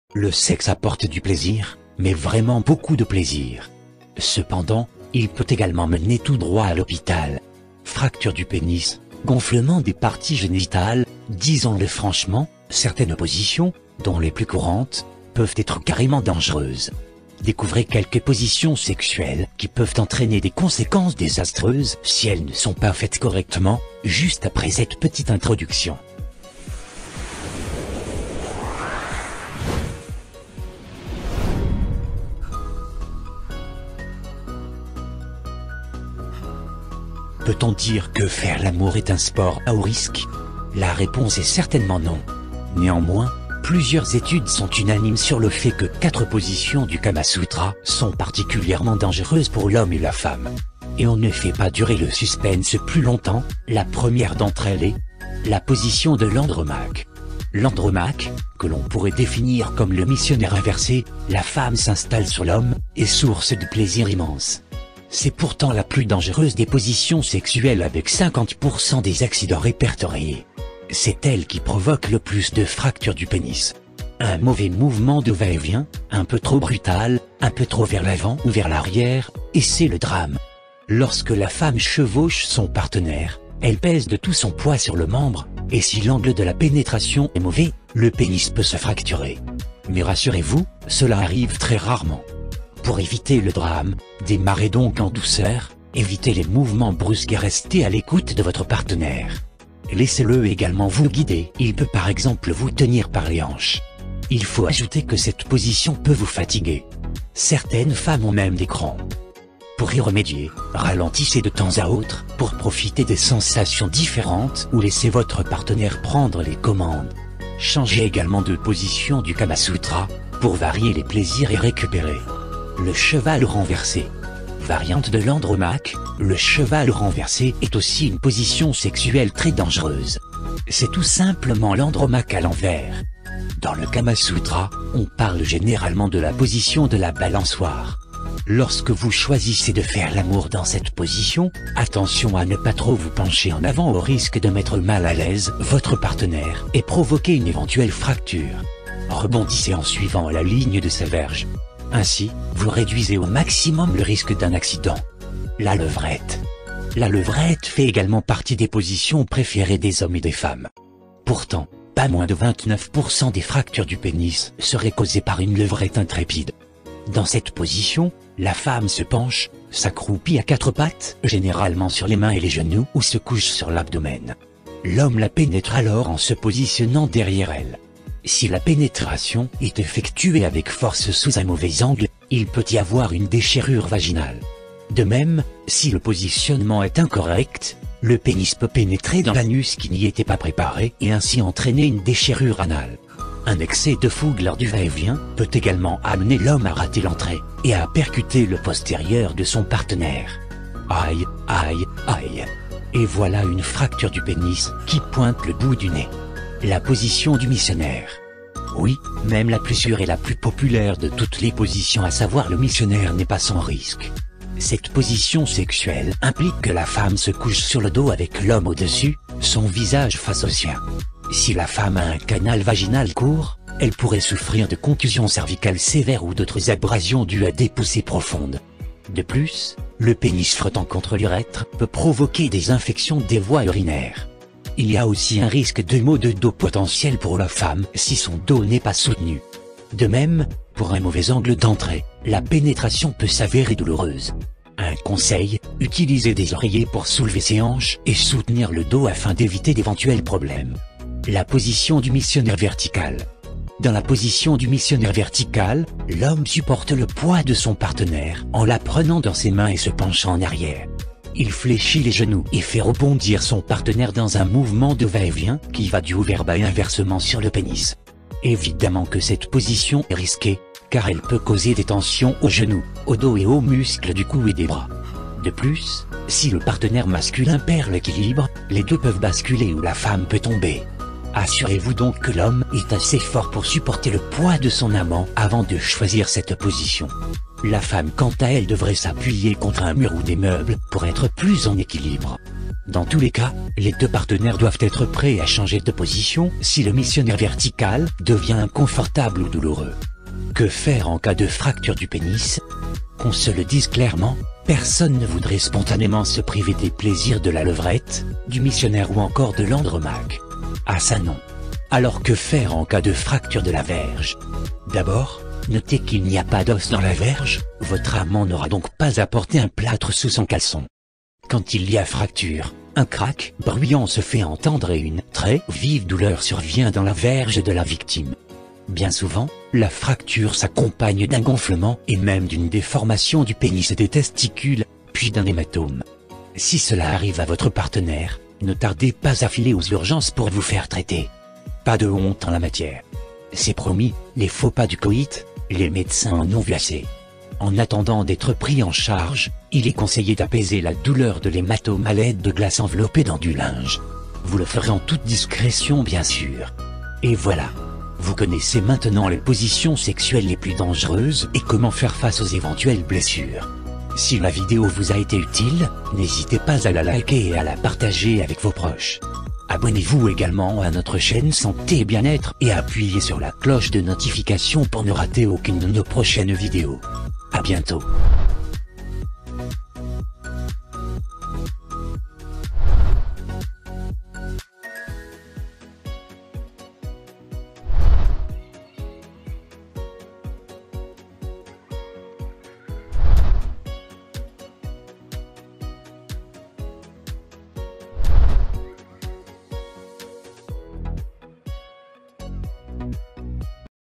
POSITIONS SEXUELLES LES PLUS DANGEREUSES
Faire l’amour, c’est plaisant. La plupart d’entre nous adorent l’idée du sexe. Cependant, il peut également mener tout droit à l’hôpital ! Fracture du pénis ou encore gonflement des parties génitales… les accidents qui peuvent survenir lorsqu’on pratique mal certaines positions sexuelles sont nombreux. Dans cette vidéo, découvrez les inconvénients de ces positions sexuelles considérées comme les plus dangereuses. Évidemment, on vous explique également comment éviter d’être victime d’accidents avec ces positions et comment y faire face lorsqu’il survient.

👉 Que faire en cas de fracture du pénis  ? : 5:00